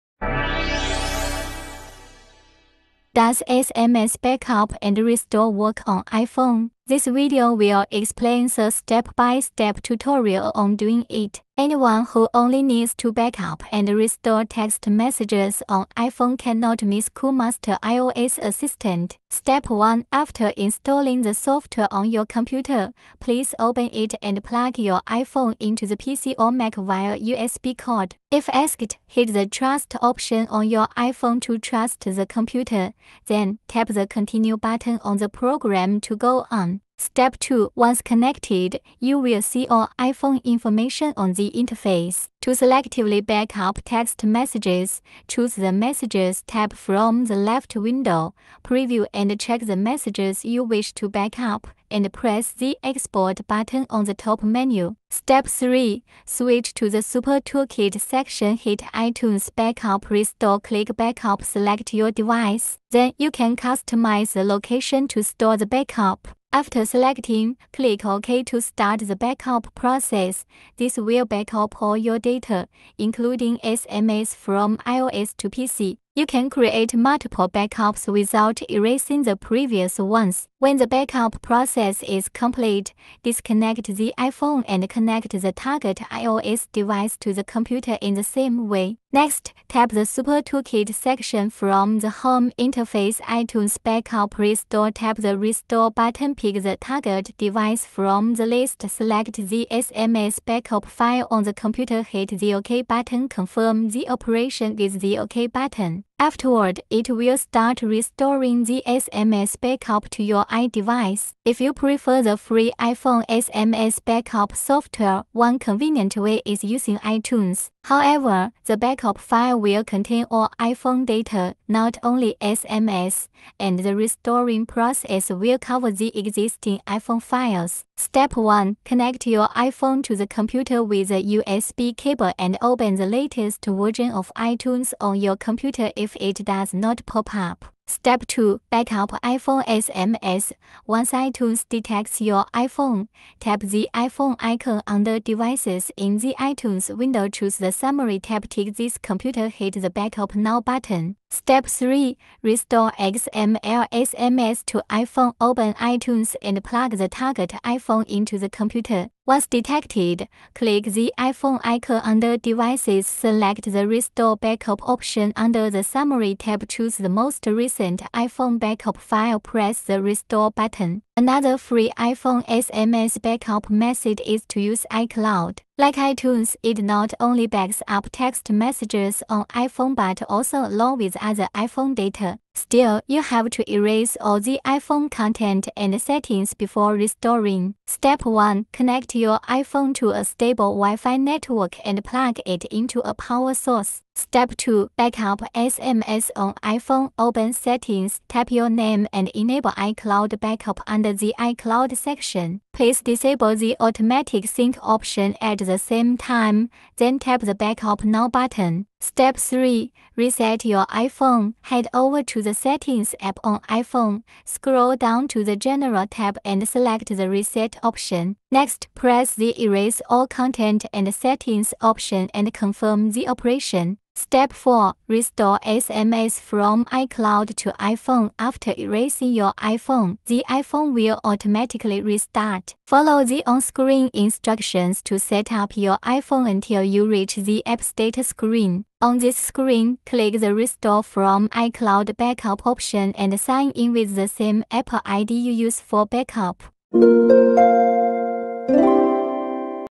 SMS Backup and Restore for iPhone: Top 3 Ways
How do I back up and restore text messages on my iPhone? Check this video to learn how to complete the SMS backup and restore on iOS devices in a few steps.